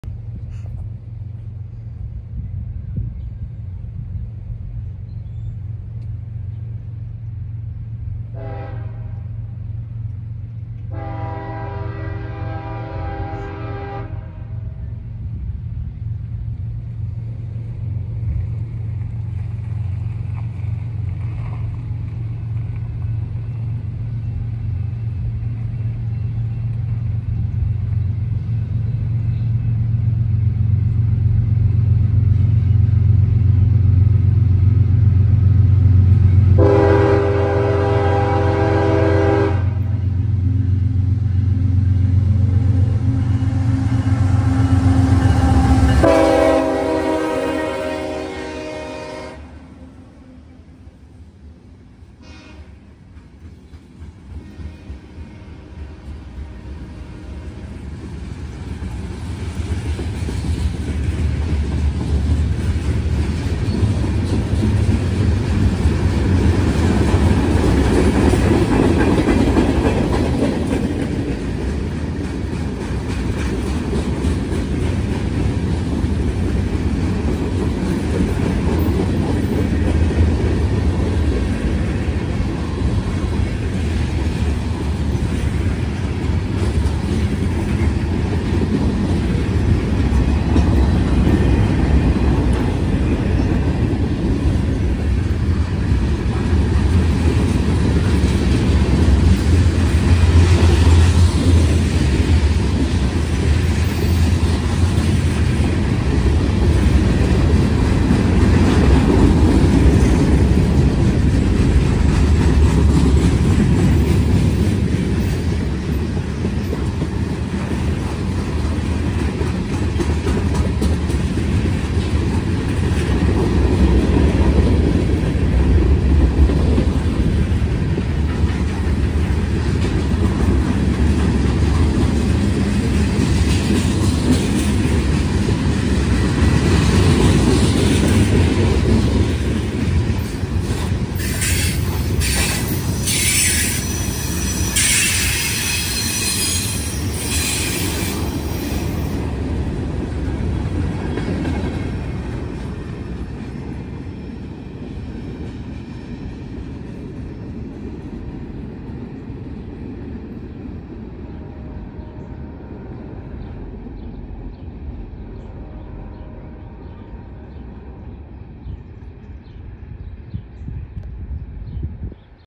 CSX Freight Train 619
Train info…
Type of train: CSX mixed freight
Power: CSX #838 + CSX #203
Symbol: Unknown
Direction: Southbound

Other info…
Location: Terre Haute, Indiana
Rail line: Intersection of CSX’s St. Louis Line & CE&D Subs
Nearby Crossing(s) and/or establishments: Filmed between 8th Avenue and Ash Street railroad crossings outside the Wabash Valley Railroad Museum.